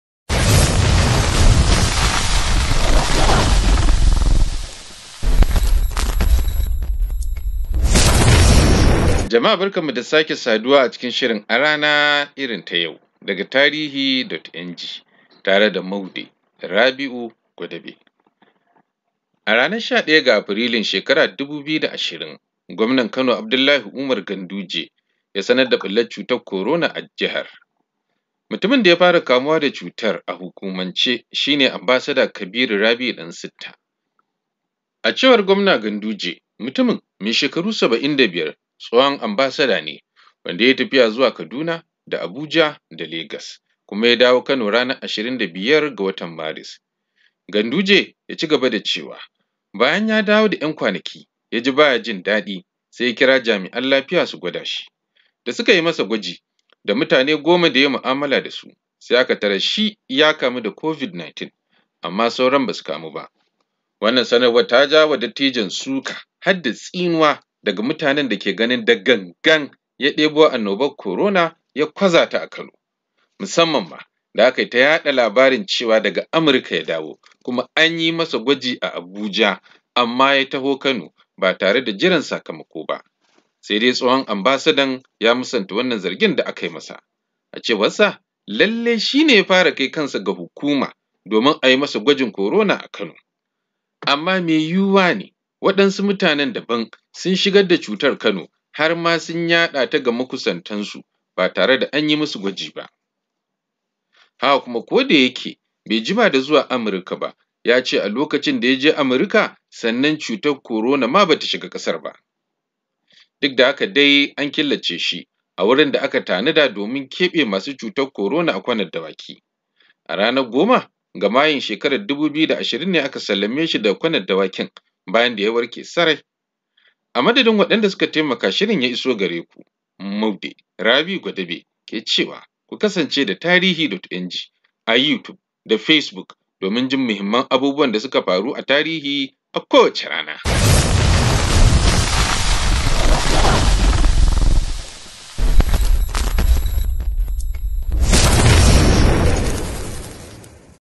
A rana irin ta yau: Afrilu 11, 2020 Covid-19 ta ɓulla a Kano
A ranar 11 ga Afrilu 2020 ne gwamnatin Kano ta sanar da Ambasada Kabiru Rabi'u a matsayin mutumin da ya shigar da cutar Korona cikin jihar.